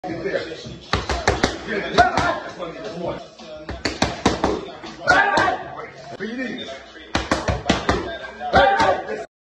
Timmy Cuamba training with Dewey Cooper
Timmy Cuamba trains with his Coach Dewey Cooper

Coach: @DCBlackKobra